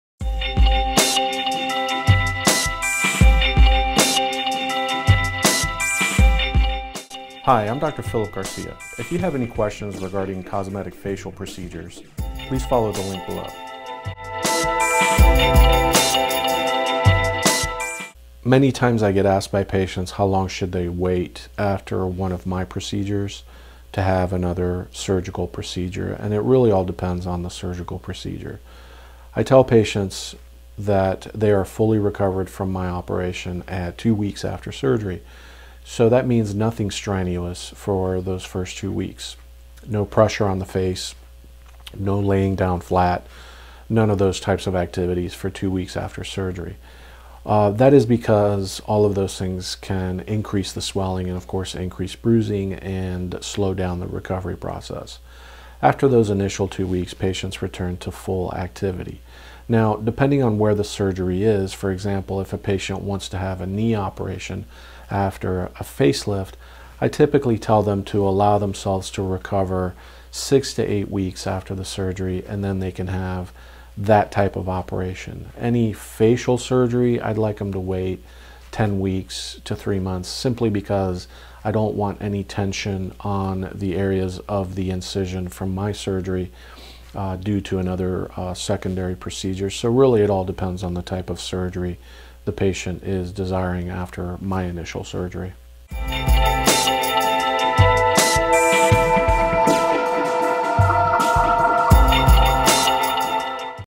Surgery After Surgery
Dr. Phillip Garcia answers the question: "How long after plastic surgery can you have another surgery?".  Many people receive multiple surgeries when they want/need a lot done. Dr. Garcia explains the recover timeline of our surgeries and how it relates to other operations.